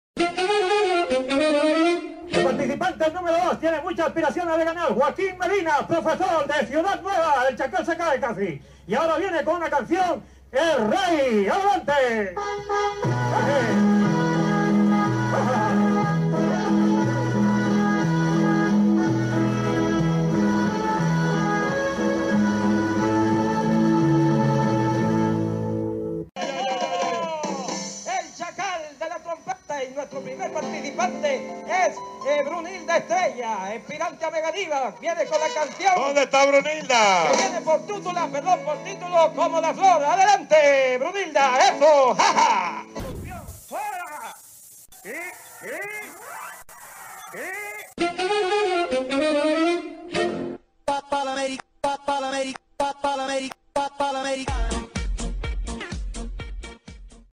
William Diaz es Don Francisco.
El imitador y humorista dominicano William Díaz, recrea y caracteriza al gran animador chileno Mario Luis Kreutzberger ""Don Francisco"".